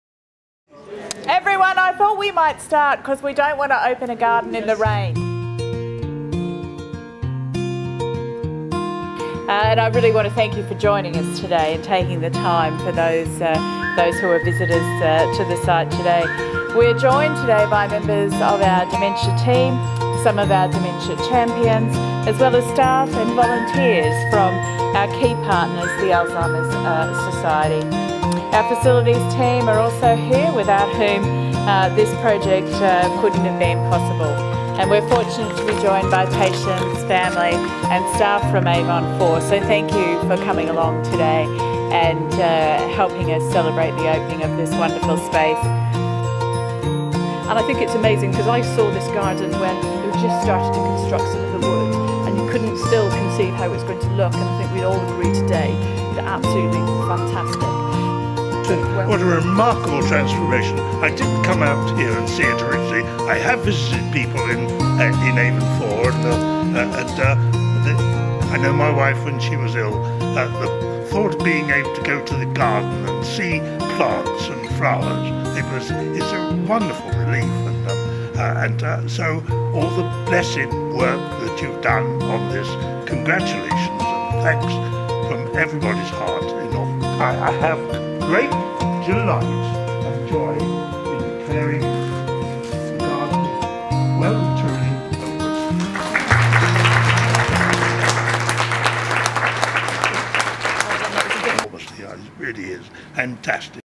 Dementia Garden opening - Worcestershire Royal Hospital
The official opening our dementia-friendly garden at Worcestershire Royal Hospital.

The sensory garden which aims to aid the recovery of patients living with dementia was formally opened by BBC Antiques Roadshow expert, Henry Sandon MBE.